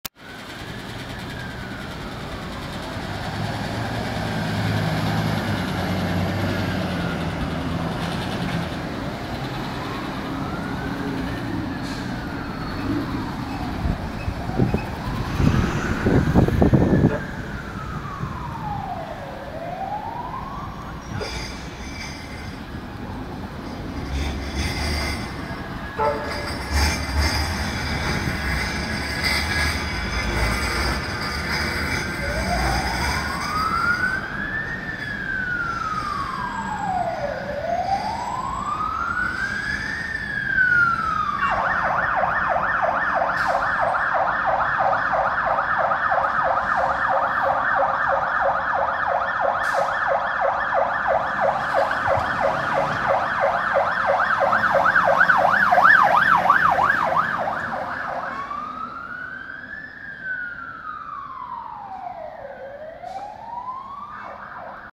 Greater Manchester Fire & Rescue - Two pumps on a shout
Greater Manchester Fire & Rescue - Two pumps on a shout in Manchester city centre